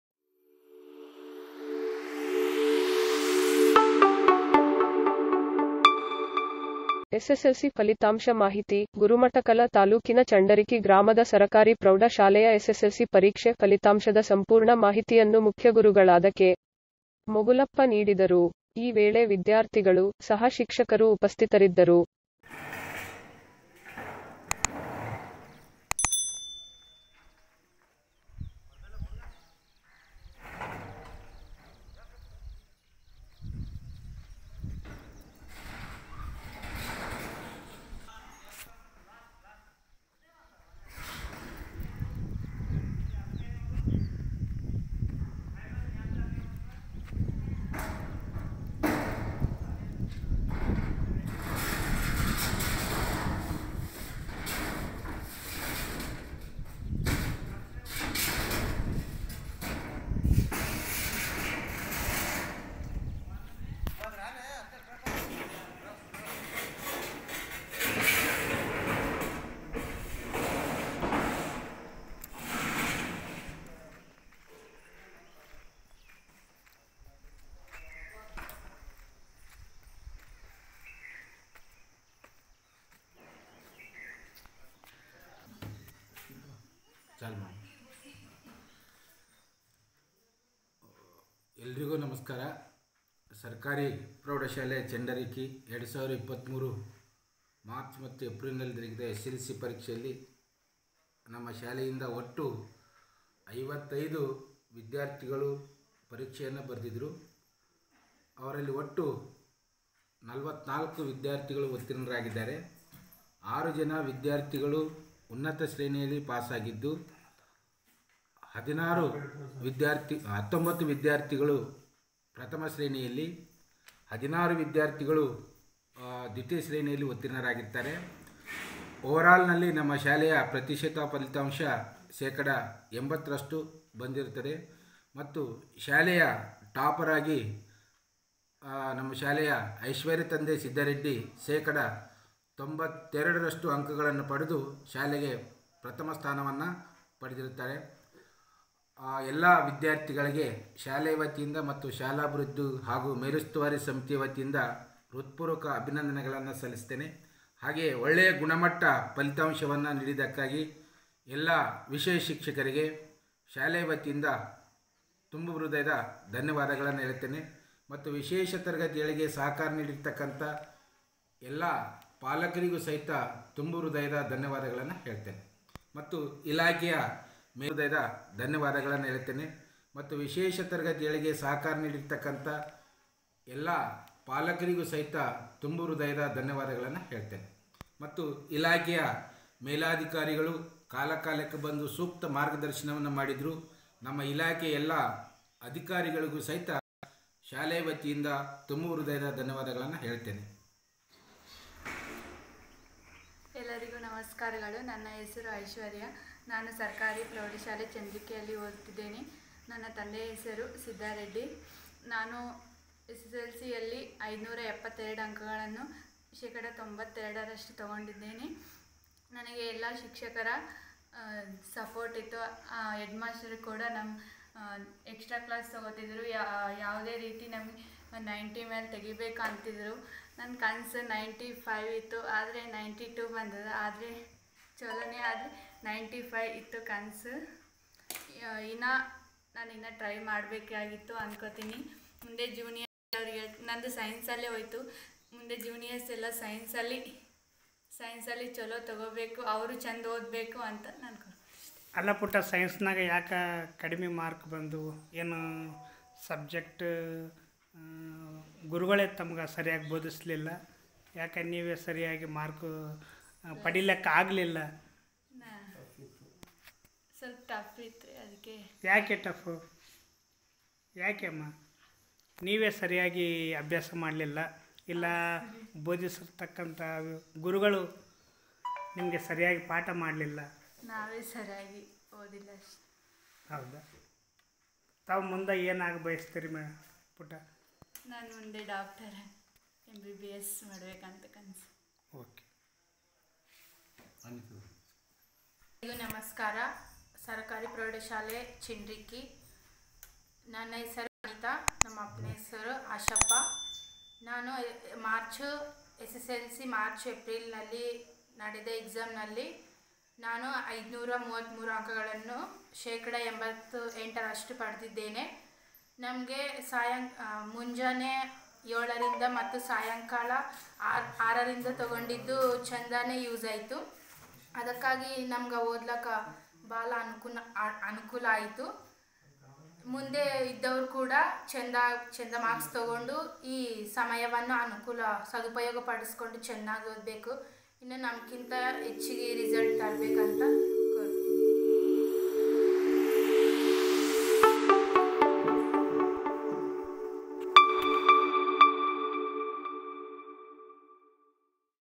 ಎಸ್ ಎಸ್ ಎಲ್ ಸಿ ಫಲಿತಾಂಶ ಮಾಹಿತಿ:
ಗುರುಮಠಕಲ ತಾಲೂಕಿನ ಚಂಡರಿಕಿ ಗ್ರಾಮದ ಸರಕಾರಿ ಪ್ರೌಢ ಶಾಲೆಯ ಎಸ್ ಎಸ್ ಎಲ್ ಸಿ ಪರೀಕ್ಷೆ ಫಲಿತಾಂಶದ ಸಂಪೂರ್ಣ ಮಾಹಿತಿಯನ್ನು ಮುಖ್ಯಗುರುಗಳಾದ ಕೆ. ಮೊಗುಲಪ್ಪ ನೀಡಿದರು. ಈ ವೇಳೆ ವಿದ್ಯಾರ್ಥಿಗಳು, ಸಹ ಶಿಕ್ಷಕರು ಉಪಸ್ಥಿತರಿದ್ದರು.